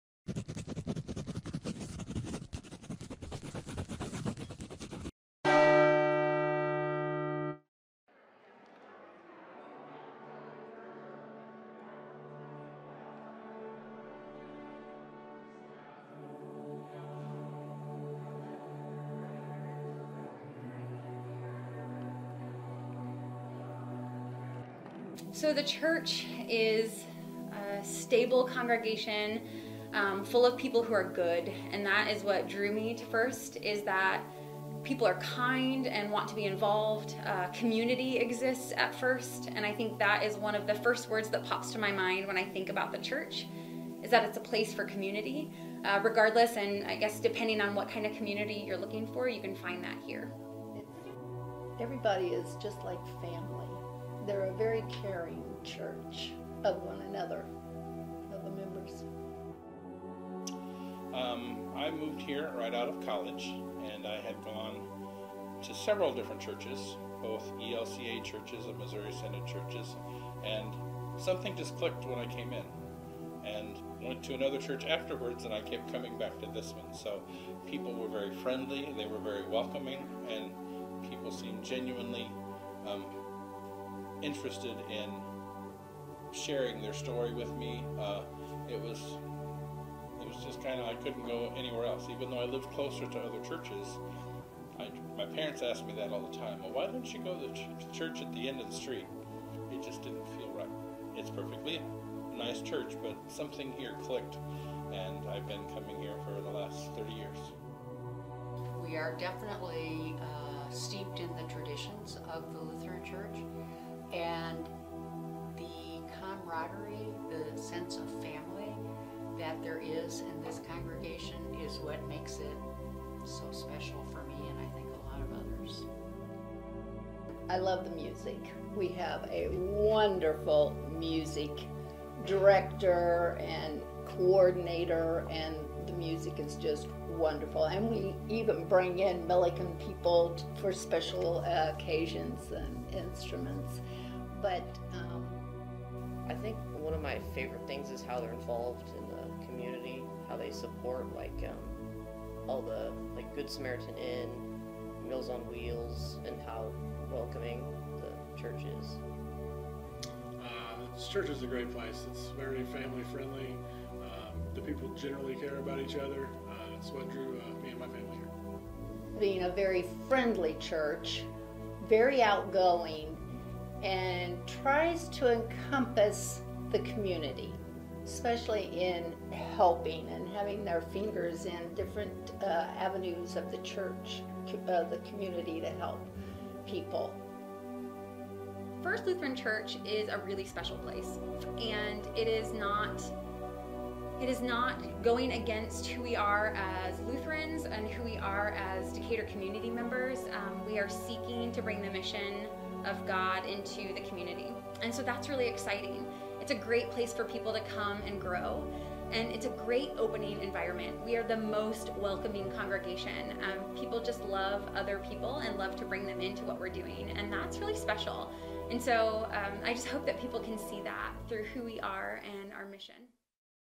FELC Get to Know Us
Get to know us video created by Millikin Creates for First Evangelical Lutheran Church in Decatur, Illinois